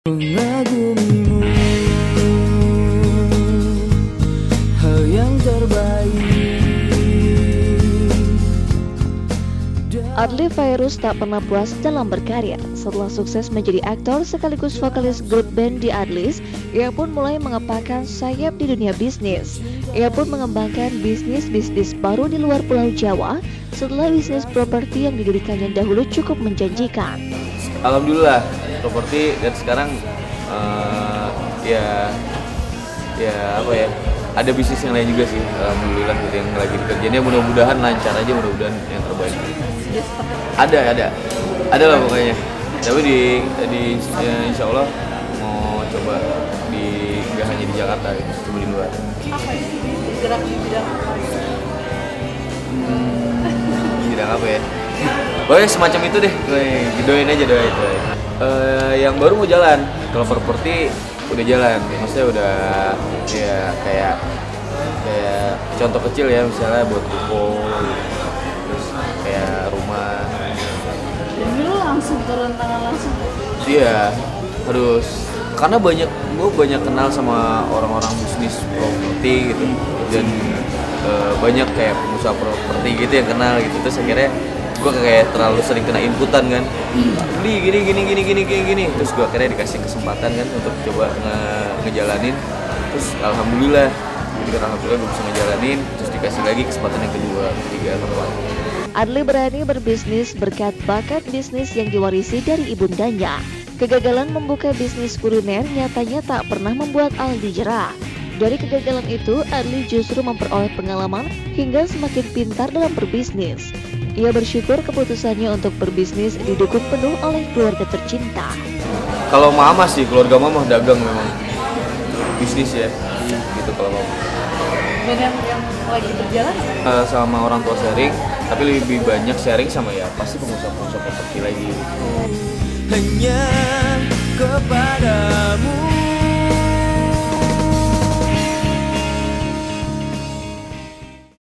Adly Fairuz Lebarkan Sayap Di Dunia Bisnis
Adly Fairuz nyatanya tak puas dengan hanya berkarier di dunia hiburan. Ia mulai melebarkan sayap di dunia bisnis. Adly bahkan mulai membuka bisnis-bisnis baru setelah sukses dengan bisnis propertinya. Bagaimana perkembangan bisnis barunya itu? Lalu seperti apa dukungan keluarga terhadap bisnisnya itu? Yuk, lihat beritanya di sini&nbsp;&nbsp; !